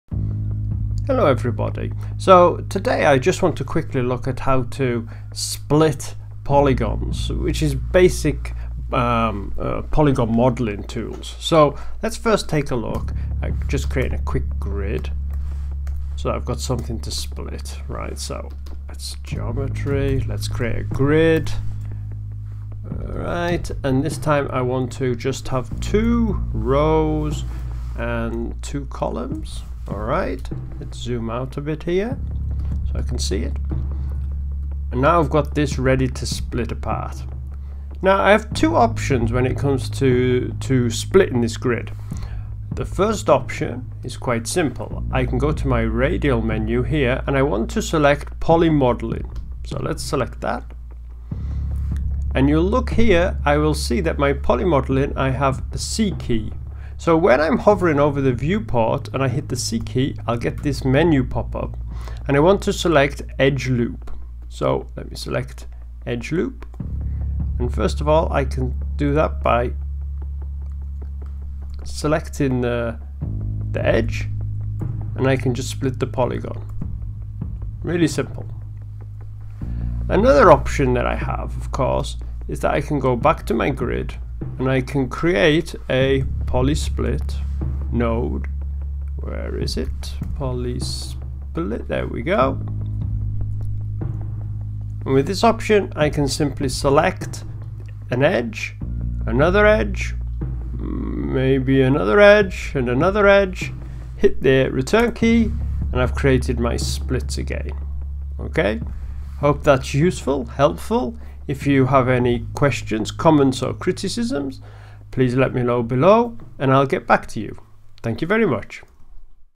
#26 Splitting a Polygon in Houdini
This tutorial looks quickly at how to split a polygon in Houdini.